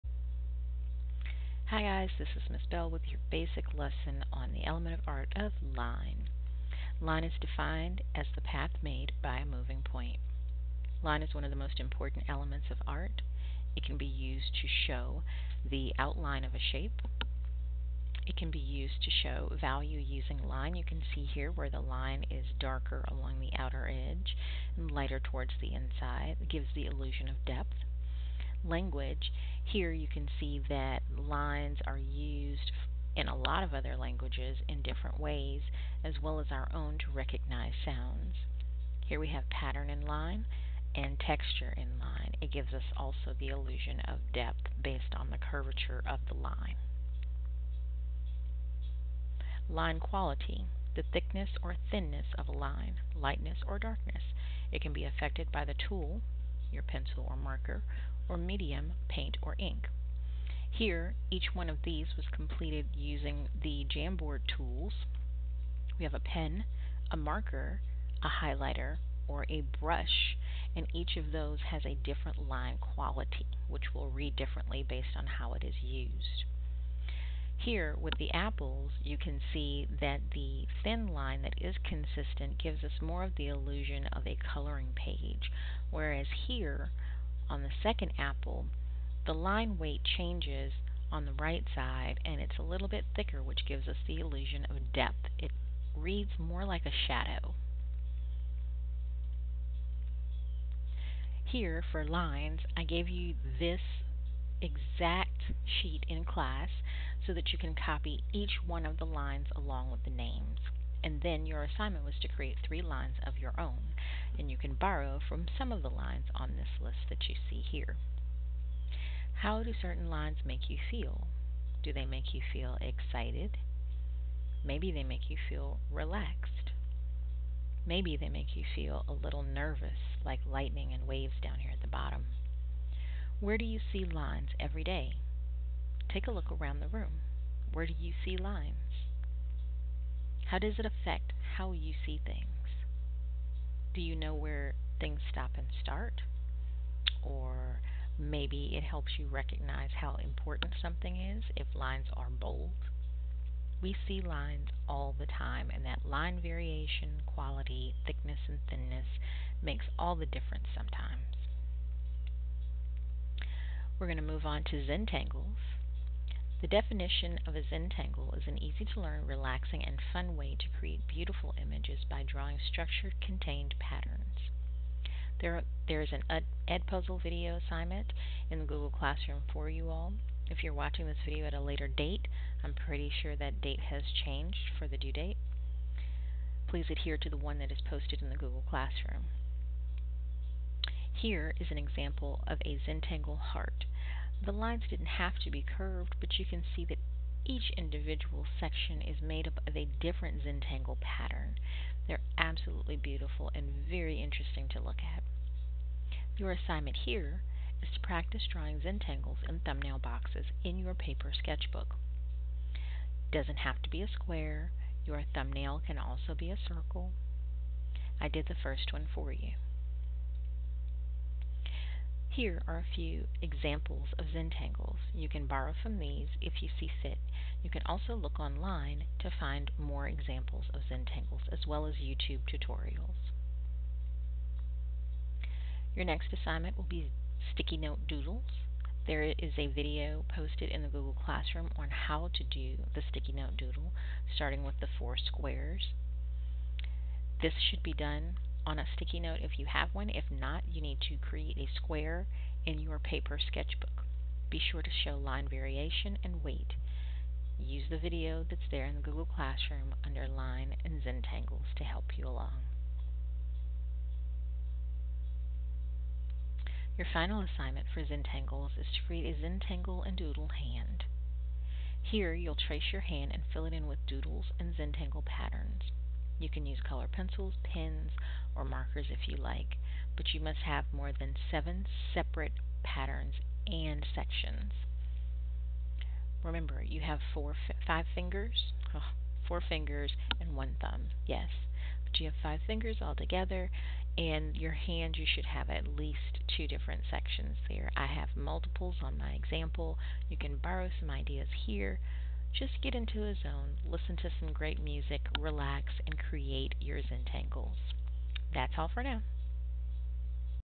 LINE LESSON PT 1- Basics & Zentangles w/ Mrs. Bell
A simple overview of the line lesson we review in class. It also lists the assignments that students will have to accompany this portion of the line lesson. *This video does not cover all things line. (the line sheet is borrowed from a former teacher)